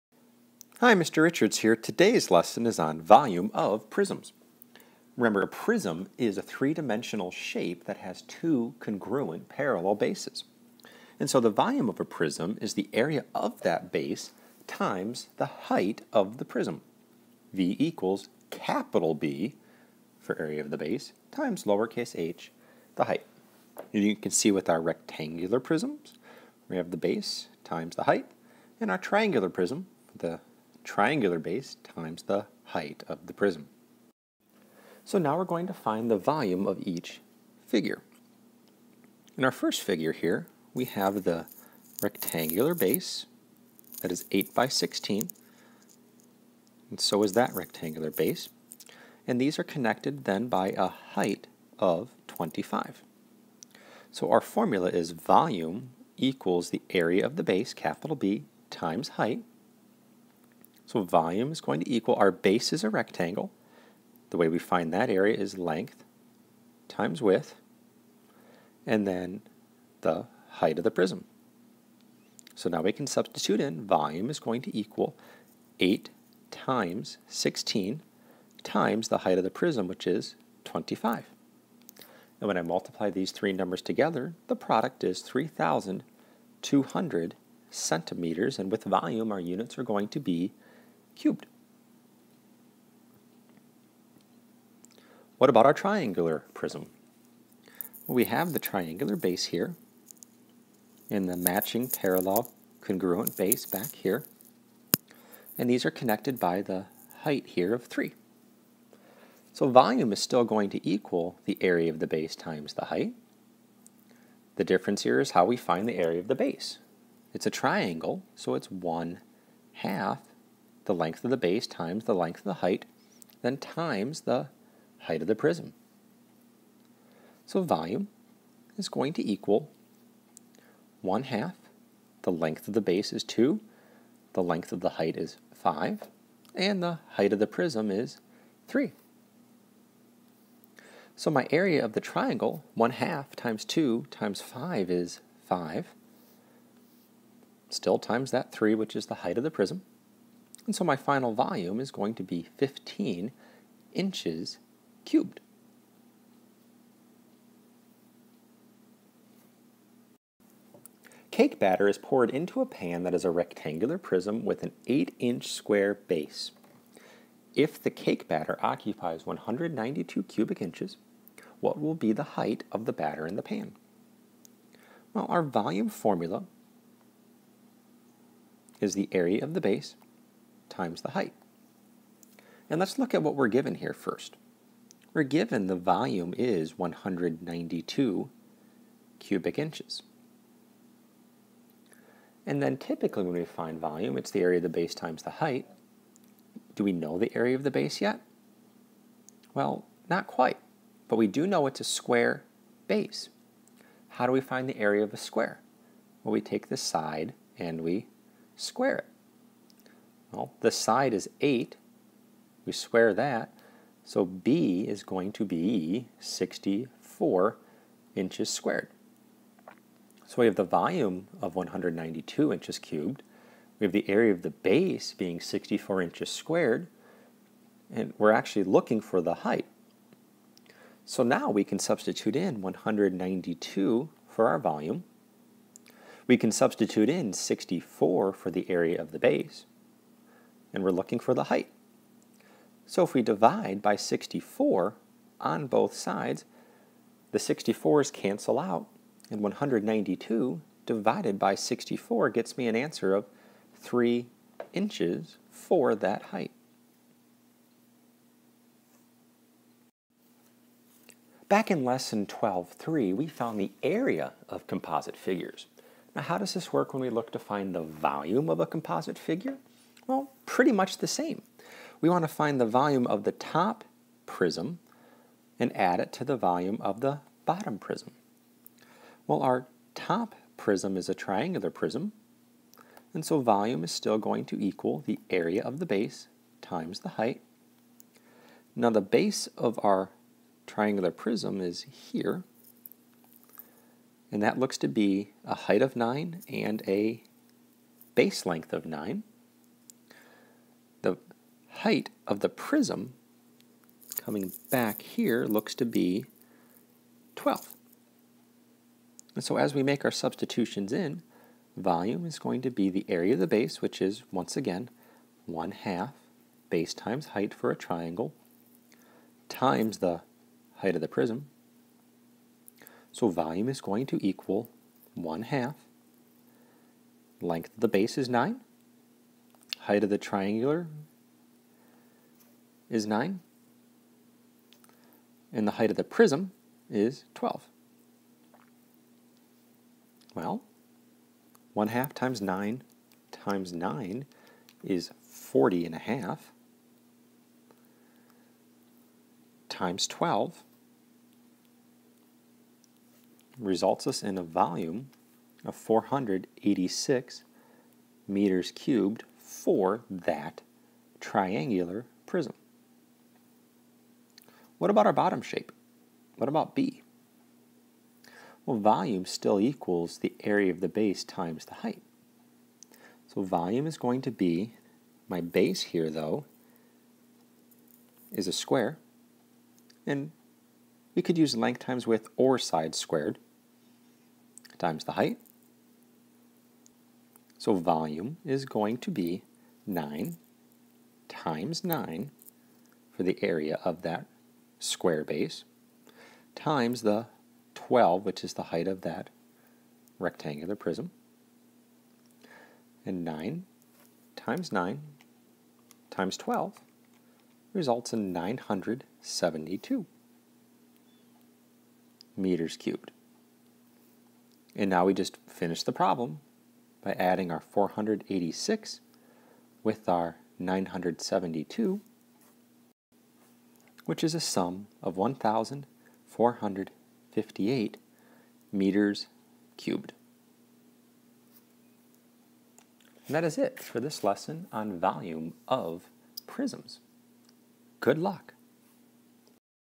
Volume of Prisms
12.5 - Volume of Prisms (Pre-Algebra)
All written notes and voices are that of Mr. Matt Richards.

Sources of examples/illustrations/pages:
8-4/Algebra I: Key Concept Boxes and Examples  © The McGraw-Hill Companies, Inc. – Carter, John A. Algebra 1. Columbus, OH: McGraw-Hill Education, 2014. Print.

8-3/Course 3 Math: Book Pages and Examples © The McGraw-Hill Companies, Inc. – Glencoe Math Course 3

7-4/Pre-Algebra: Key Concept Boxes, Diagrams, and Examples  © The McGraw-Hill Companies, Inc. – Carter, John A. Glencoe Math Accelerated. Columbus, OH: McGraw-Hill Education, 2014. Print.

7-3/Course 2: Book Pages and Examples © The McGraw-Hill Companies, Inc. – Glencoe Math Course 2